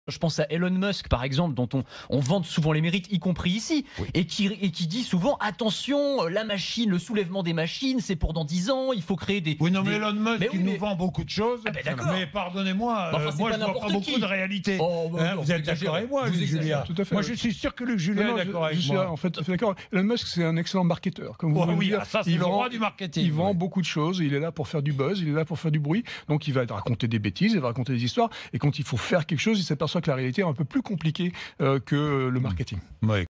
Le petit tacle de Luc Julia, vice-président de l’innovation chez Samsung, à Elon Musk
"Il est là pour faire le buzz, donc il raconte des histoires, mais..."